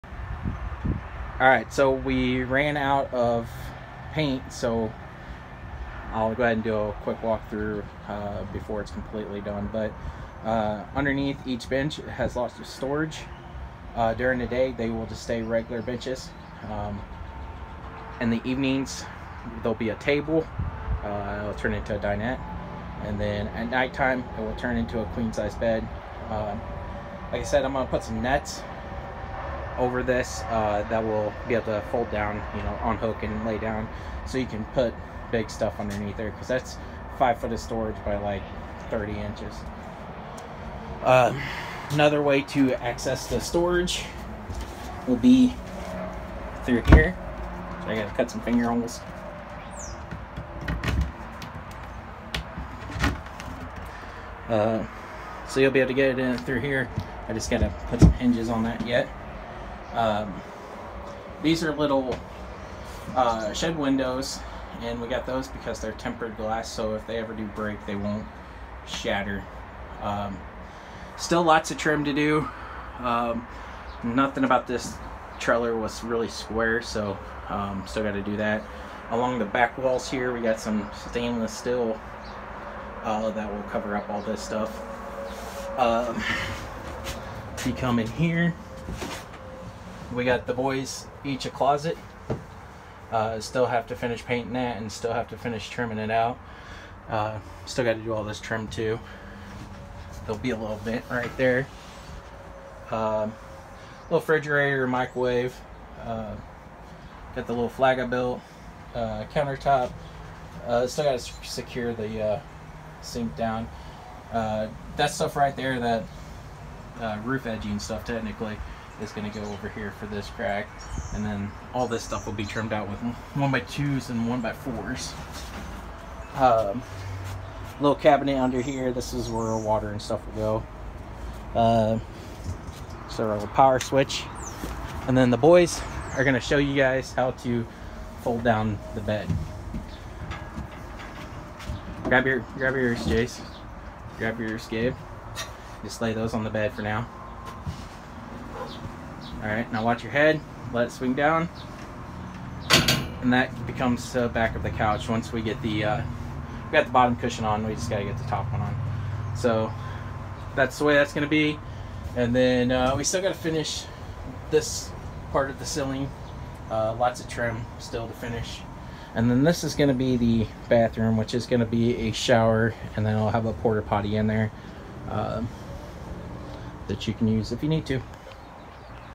7x12 cargo trailer conversion
My 7x12 mid build tour.